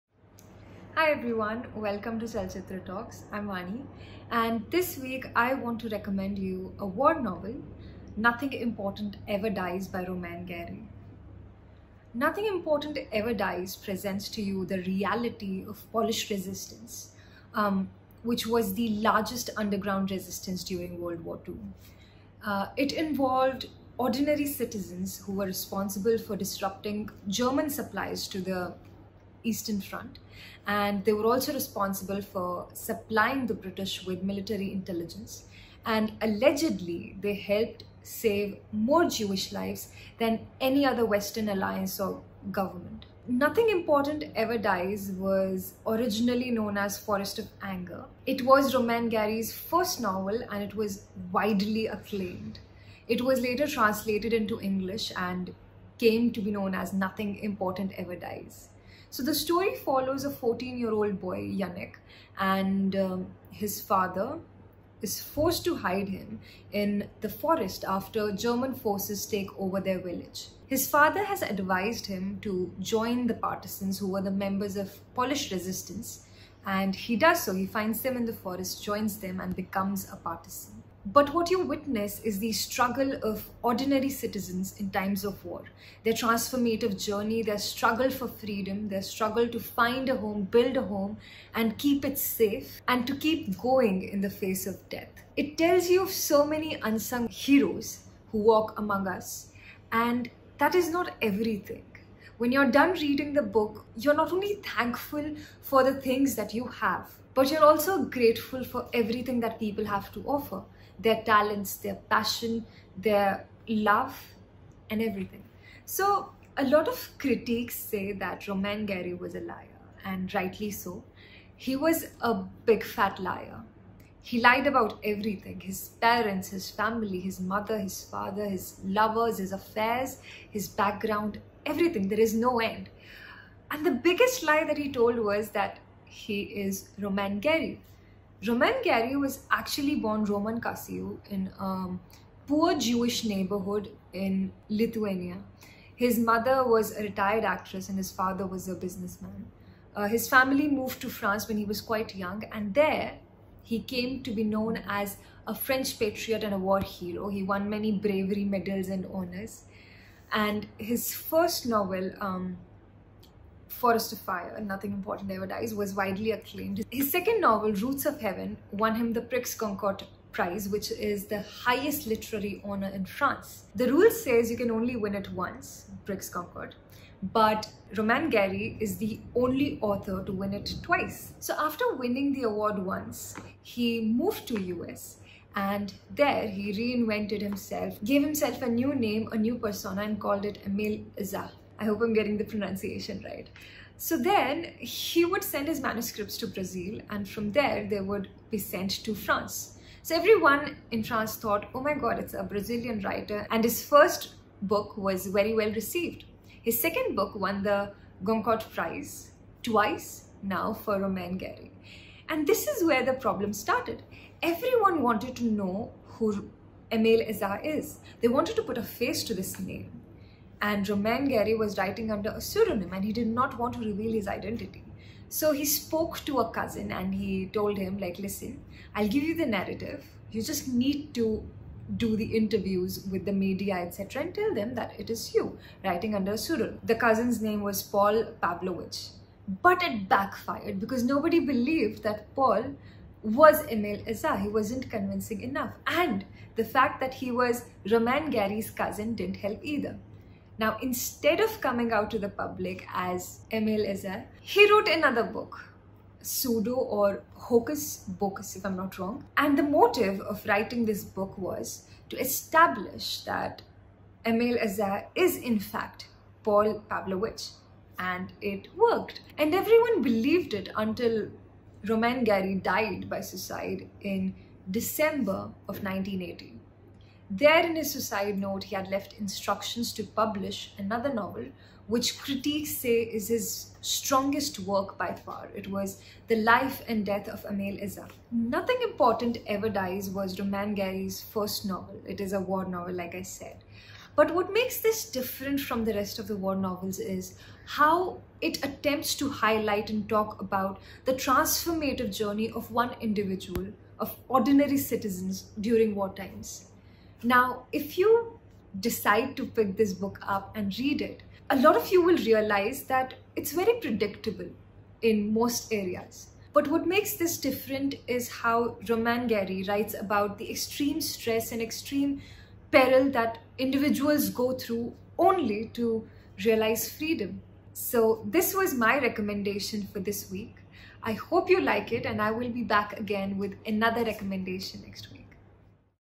Ep 12 | Book Recommendations | Kitaab Ghar | Discussing About Romain Gary
In this series, we'll be recommending books to read every week.

Book recommended : Nothing Important Ever Dies by Romain Gary

Channel run by : Vaibhav Munjal & Sorabh Munjal

You can now directly contribute to us on Youtube by becoming a part of the community :